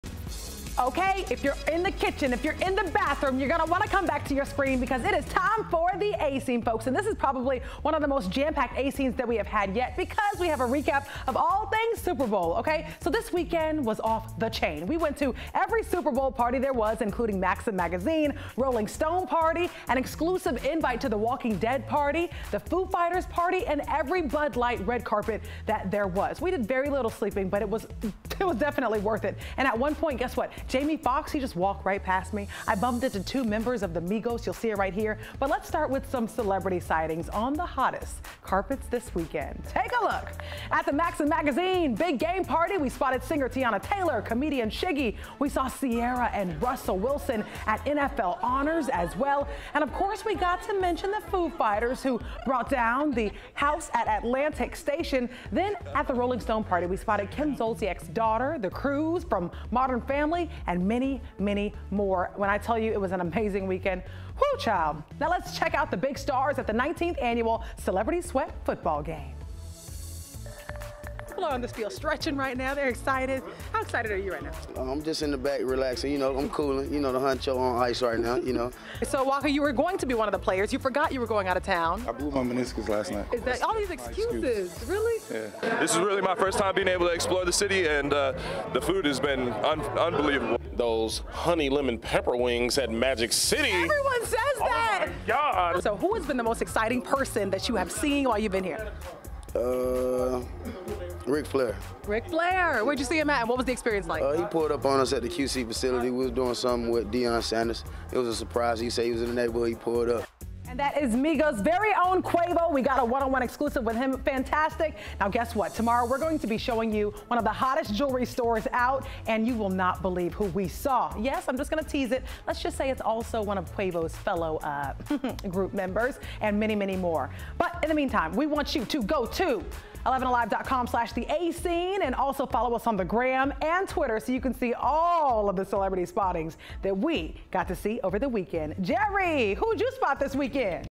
Maxim, Rolling Stone among the hottest Super Bowl 2019 parties
Super Bowl 2019 weekend was off the chain. A-scene met with some big names this weekend.